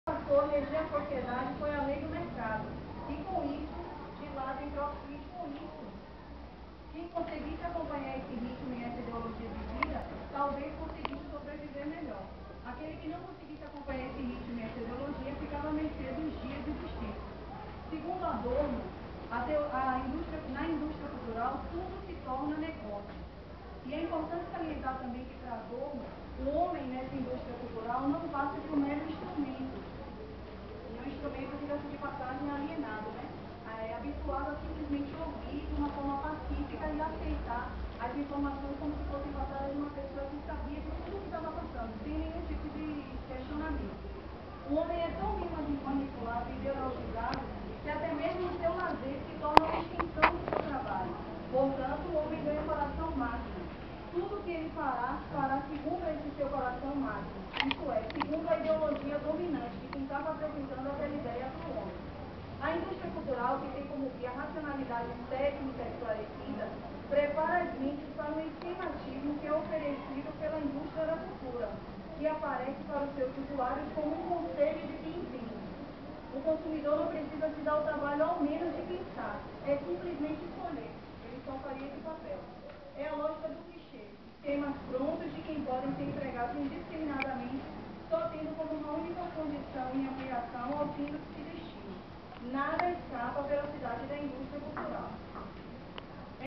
Jornalismo , Apresentação de trabalho
Em aula da Faculdade da cidade, Jaqueline Bonatii apresenta trabalho sobre teoria da Comunicação.